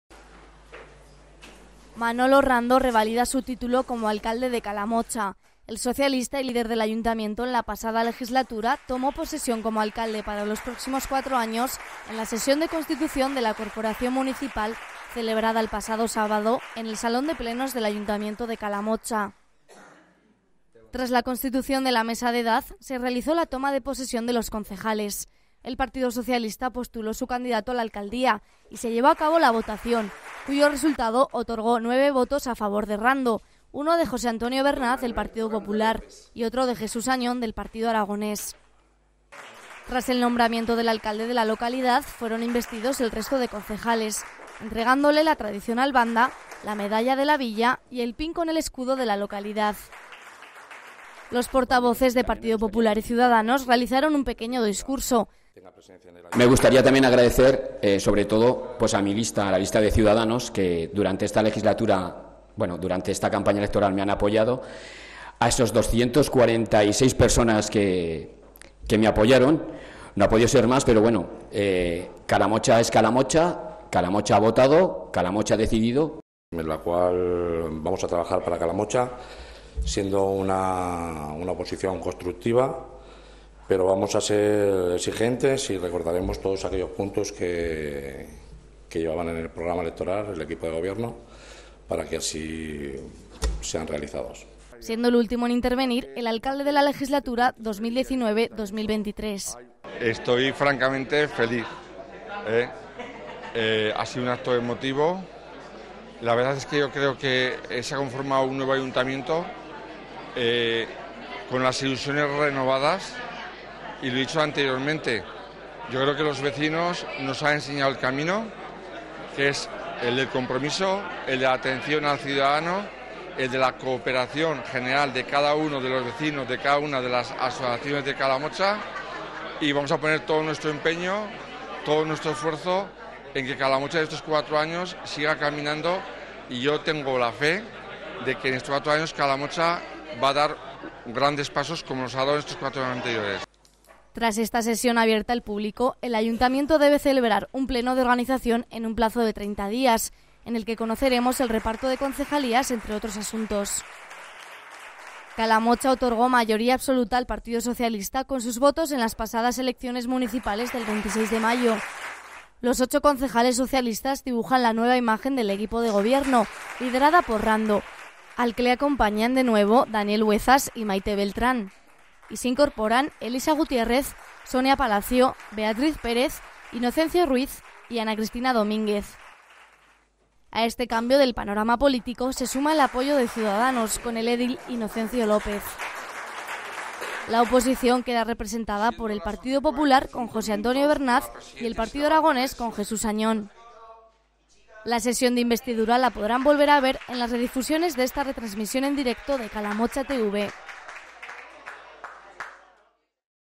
Manolo Rando revalida su título de alcalde de Calamocha
Manolo Rando tomó posesión como alcalde de Calamocha en la sesión de constitución de la nueva corporación municipal.